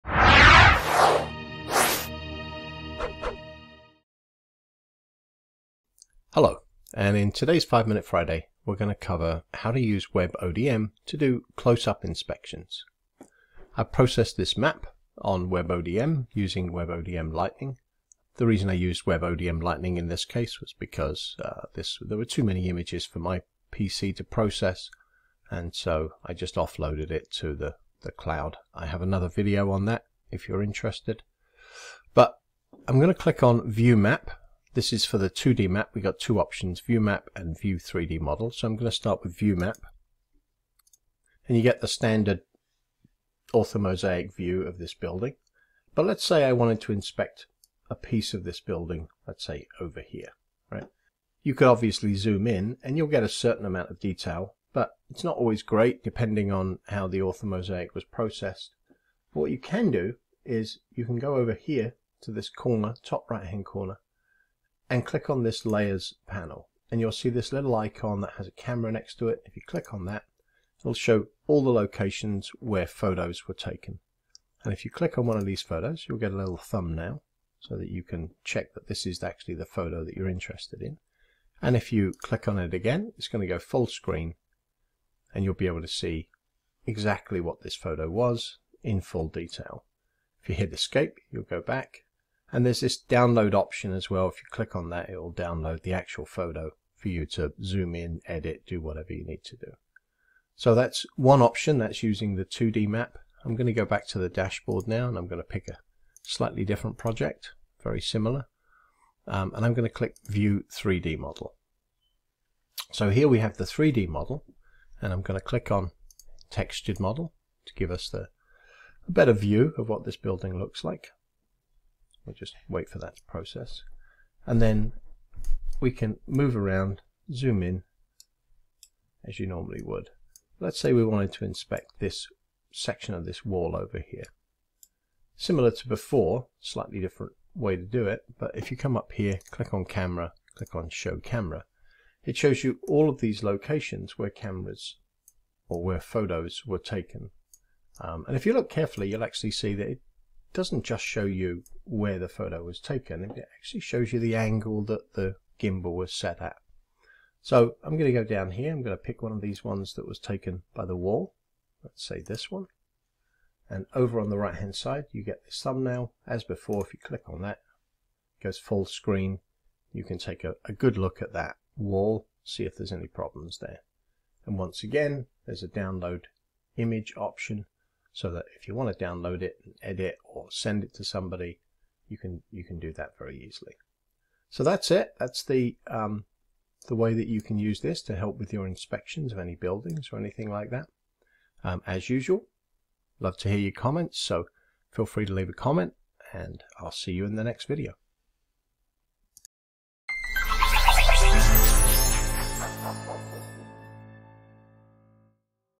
Using WebODM for Inspection
WebODM does a great job of showing orthomosaic and 3D renderings.  But if you really want to look at something closely you are going to want to look at the original photos.  This method shows how to use WebODM to find just the photo that you need!

WebODM is based on Open Drone Map

Avios Media offers drone automation consulting, and video production for Commercial Real Estate (CRE) and Construction Projects